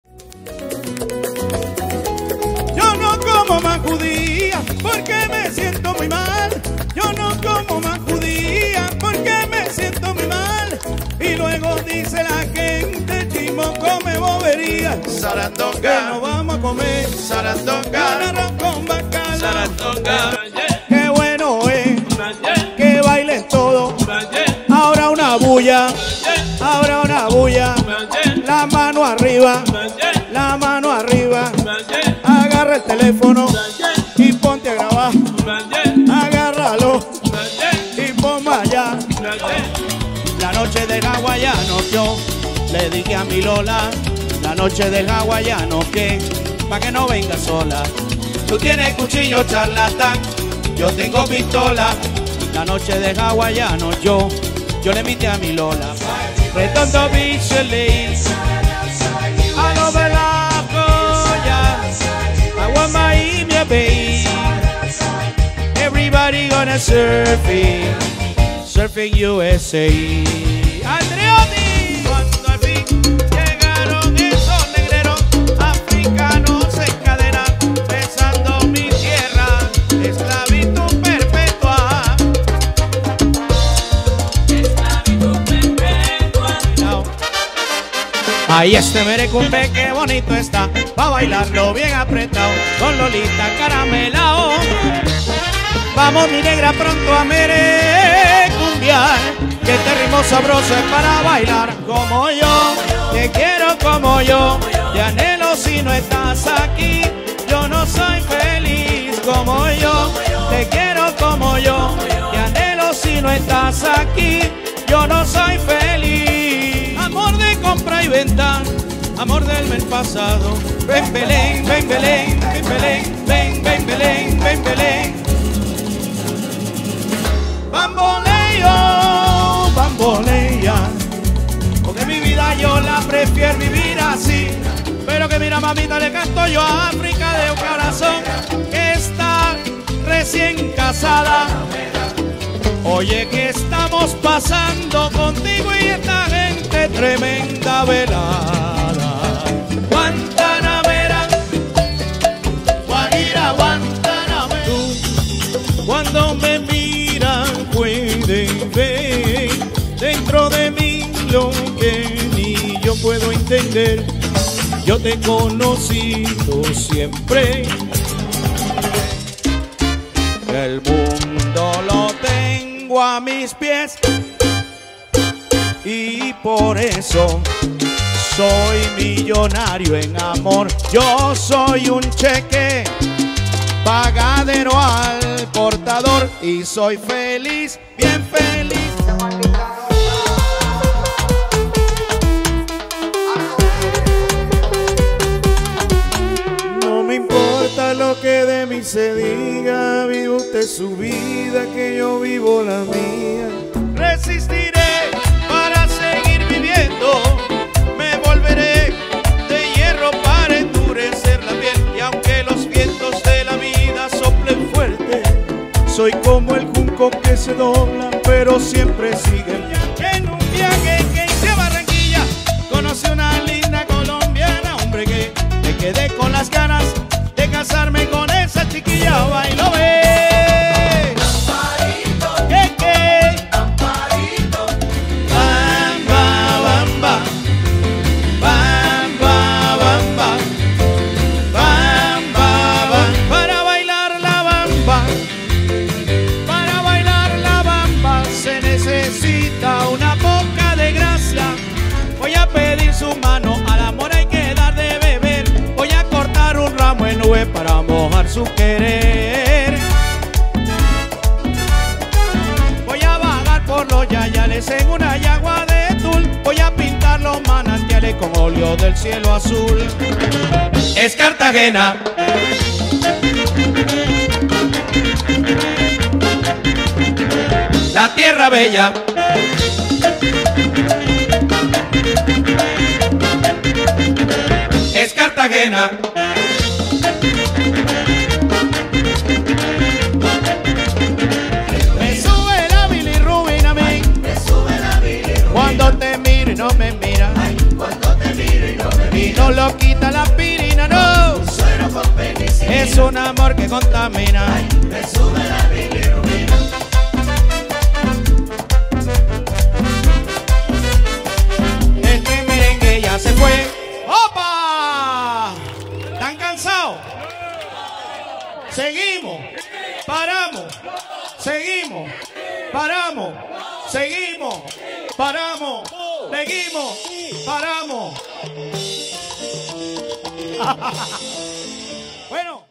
The Latin Ensemble Live! Show en Segovia, España
Resumen de una supuesta hora coctel... que terminó siendo un baile!